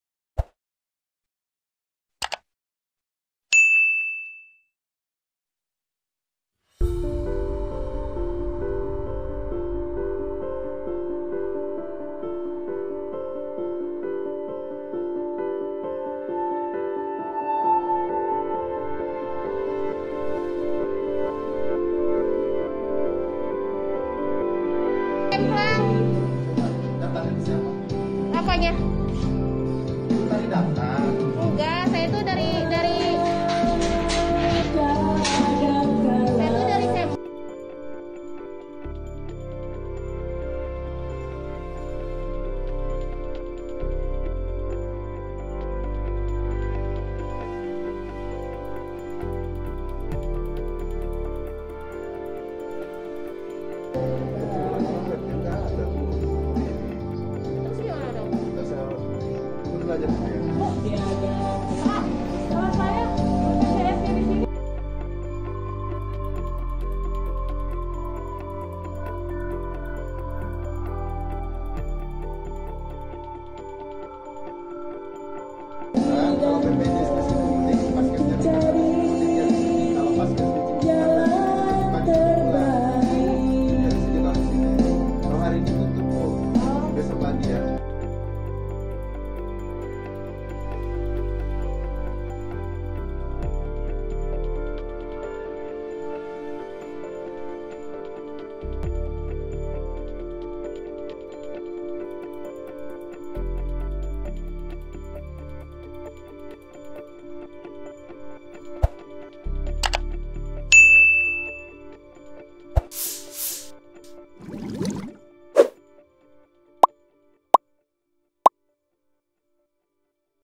Heboh, Petugas Puskesmas Asik Karaokean saat Jam Kerja, Tolak Warga Positif Covid 19
Sebuah video memperlihatkan warga yang mengaku positif covid-19 mendatangi Puskesmas di Bogor, Jawa Barat.

Saat berniat berobat, Puskesmas tersebut tutup saat jam kerja dan para petugas Puskesmas itu sibuk karokean.

Video ini dibagikan oleh akun Twitter @txtdaribogor pada Jumat (9/7/2021).

"Puskesmas Desa Situ Udik pada jam kerja seharusnya melayani pasien yang hendak ingin berobat malah tutup dan sibuk asik karokean," tulis akun Twitter @txtdaribogor

Video terkini dan terbaru..